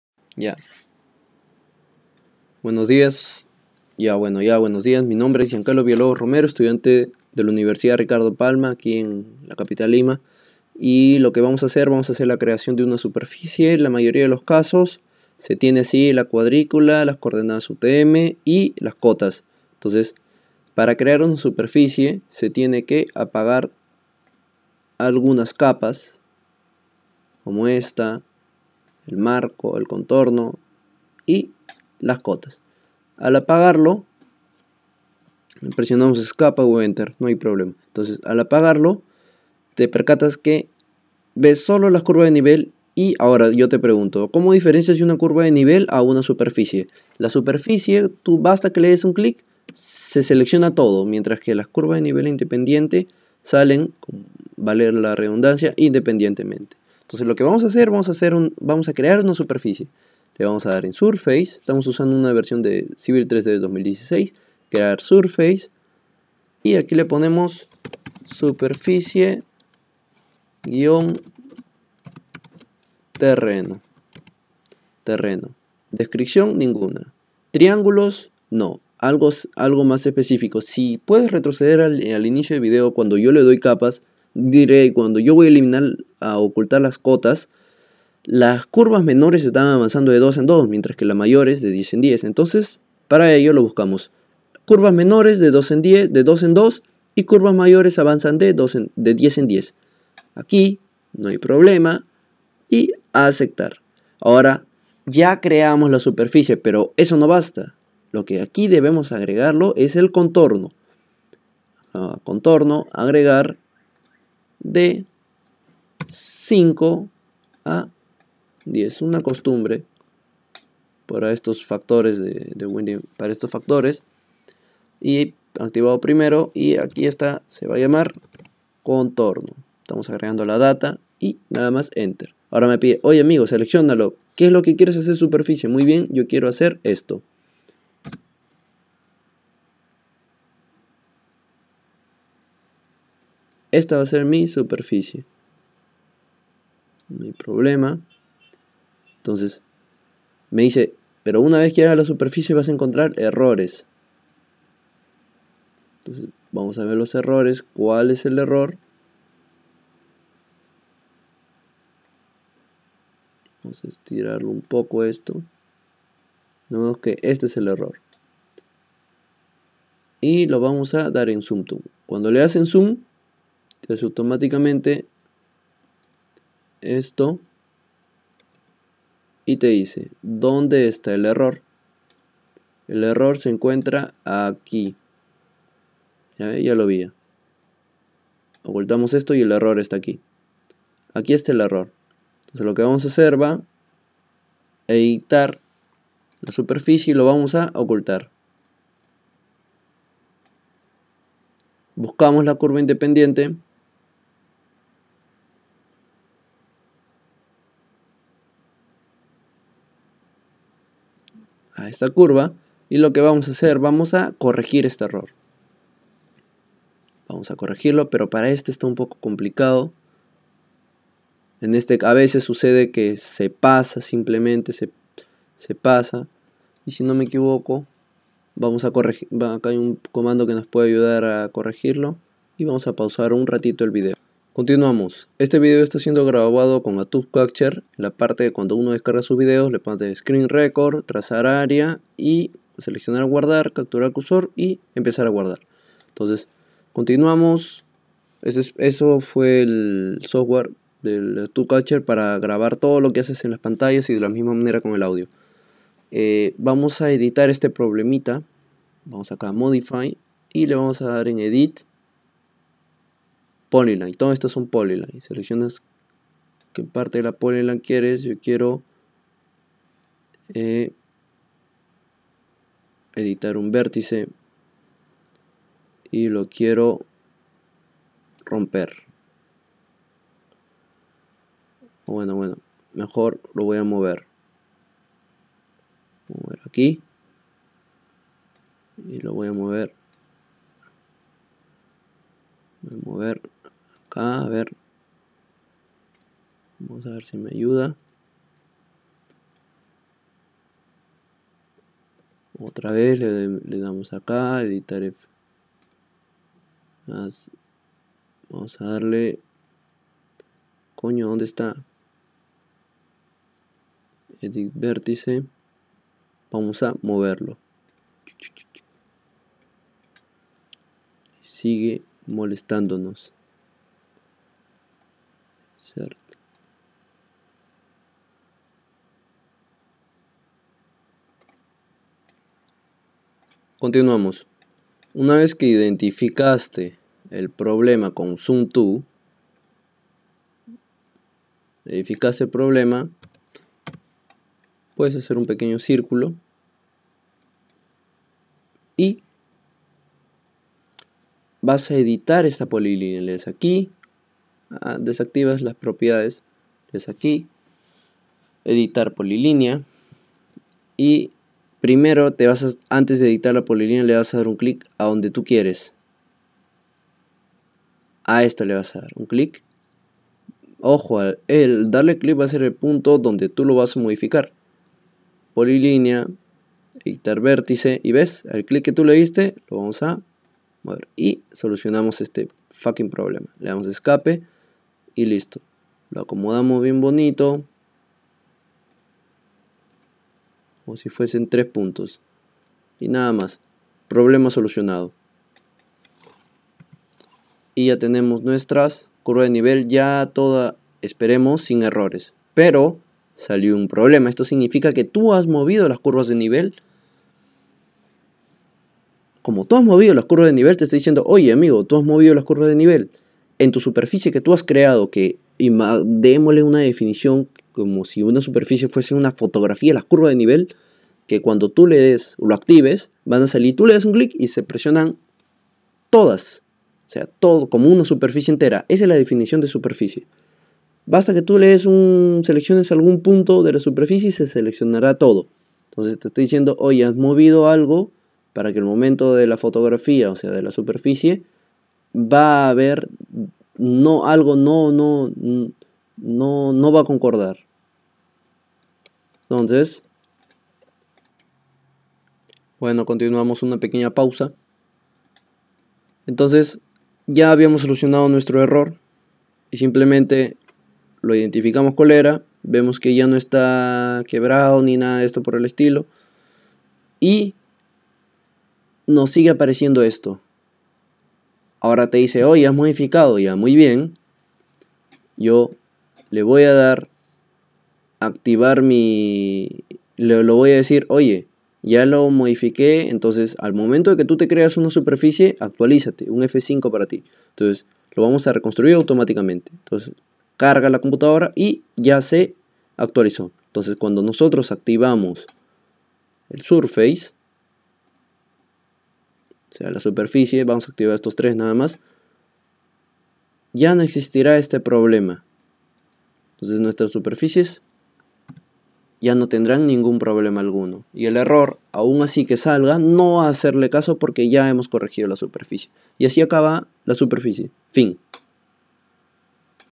TUTORIAL / Video 2.1 - Corrección de superficies triangulares (PART 1/2)
(PARA VER LA APLICACIÓN DE LA PLANTILLA Y POSTERIORMENTE LA EJECUCIÓN DEL PROYECTO - VER VIDEO 1)

Manual de Diseño Geometrico de Vias 2014 (Norma Actualmente usada 2017) PDF del Ministerio de Transportes (MTC) Perú.

MENTORES:
Ing. Xavier Garfias Zúñiga.
Ing. Luis Ponce Filios.
Ing. Mercedes Rodríguez Prieto.

*CUALQUIER DUDA O CONSULTA COMÉNTALO PARA AYUDAR A TAMBIÉN A LOS DEMÁS. SALUDOS.